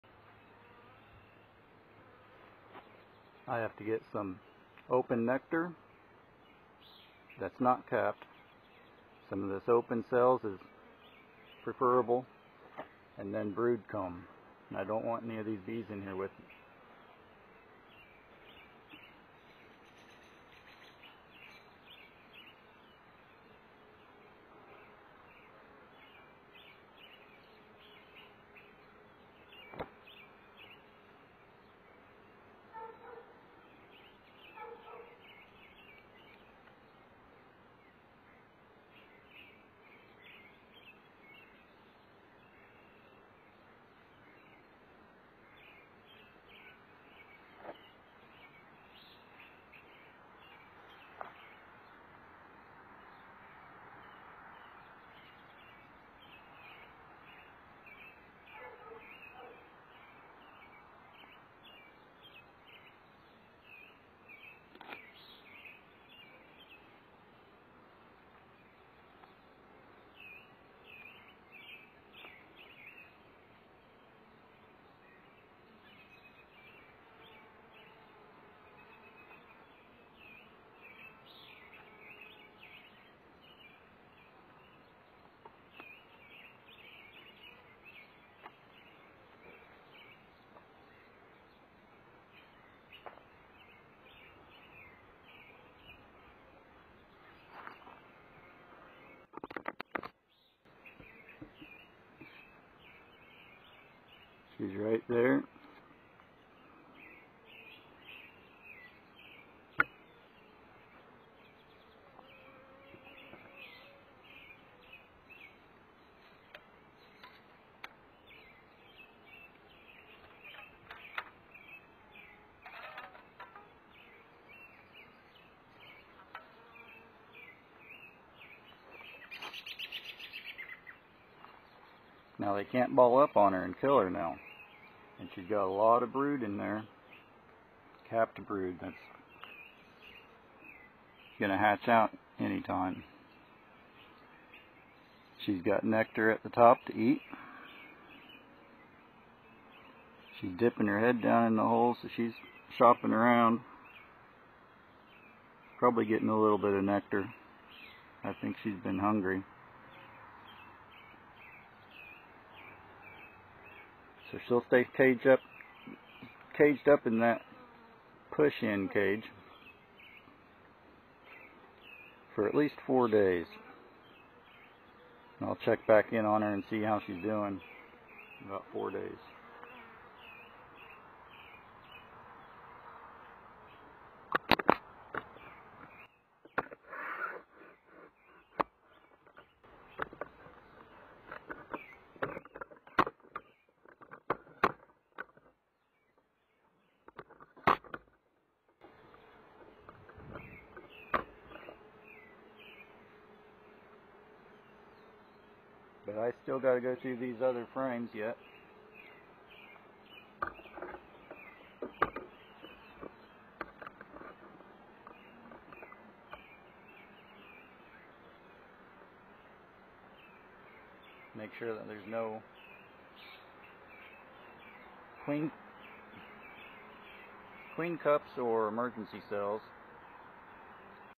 introducing queen bee with pushin queen cage
This shows part of a hive split. The split was done 3 days prior to age any eggs in the hive to the point of not viable for queen rearing. A few eggs were in the process of becoming emergency cells which I removed. Beyond 3 days, they cannot make the old eggs into queens so it is then ready to introduce a queen. This method is the safe way, albeit a bit overkill, to introduce a queen. After 4-5 days, the cage will be filled with hatched nurse bees depending on how many capped brood you caged. These nurse bees are instantly accepting of their new queen and take care of her for the caged duration. The wooden cages do not allow the queen to lay eggs for the caged duration, there for, she may still not be accepted when you finally release her. This method will result in fresh eggs within the 4 days and her eggs will fill the hive with her scent, making acceptance more likely.